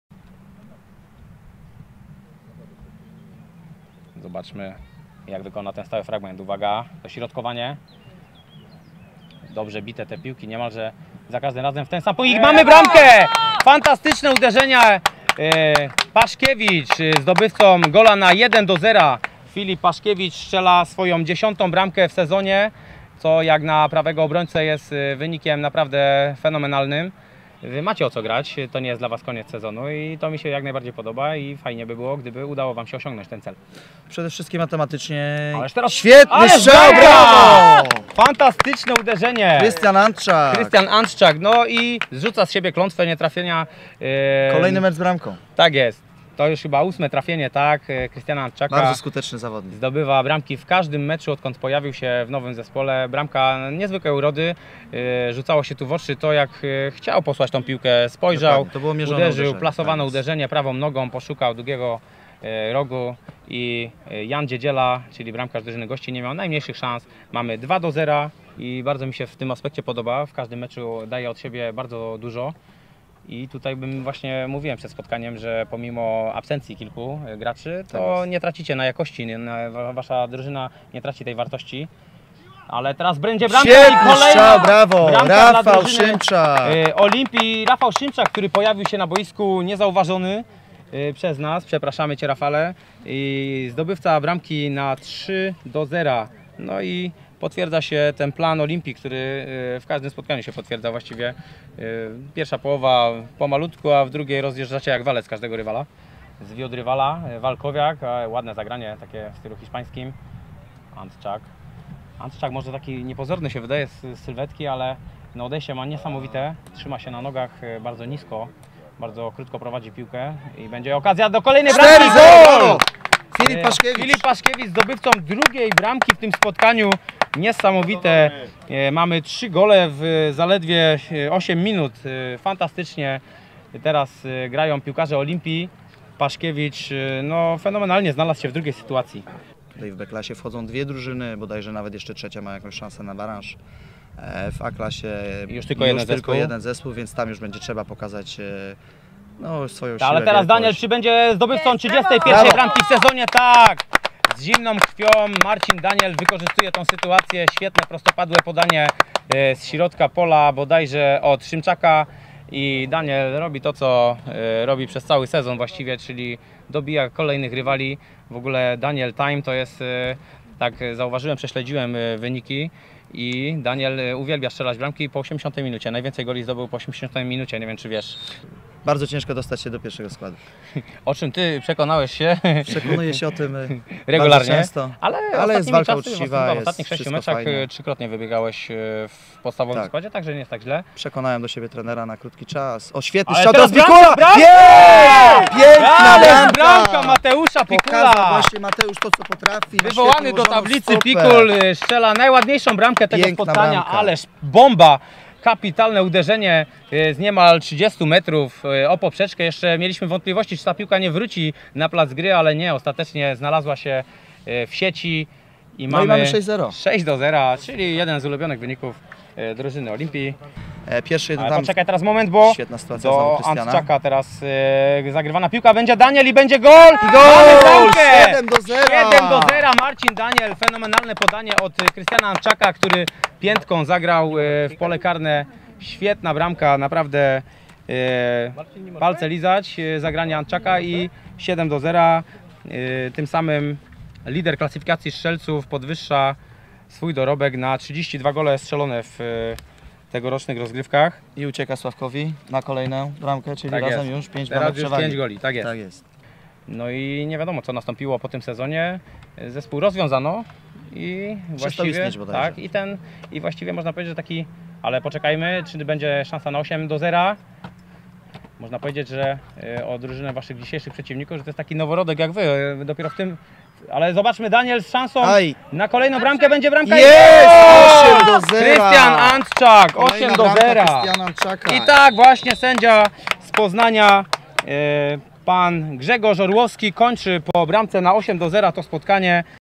Skrót meczu: IKP Olimpia Poznań 8-0 Błękitni Owińska
Zobacz bramki i skrót meczu gr. 1 B Klasy, IKP Olimpia Poznań 8-0 Błękitni Owińska. Gole dla Olimpii strzelali Krystian Antczak x2, Marcin Daniel x2, Filip Paszkiewicz, Rafał Szymczak i Mateusz Pikul.